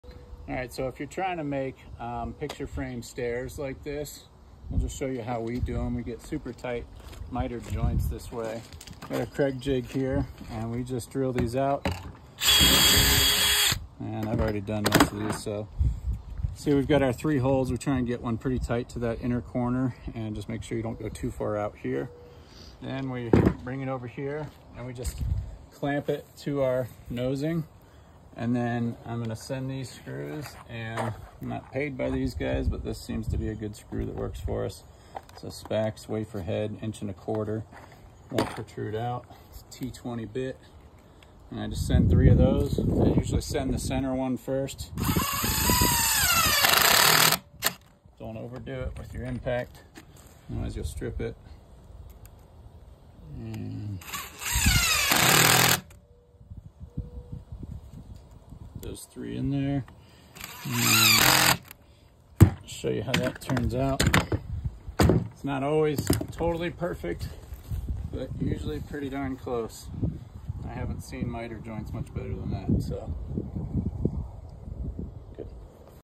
Miter trex deck boards
How to accomplish perfect miters with composite decking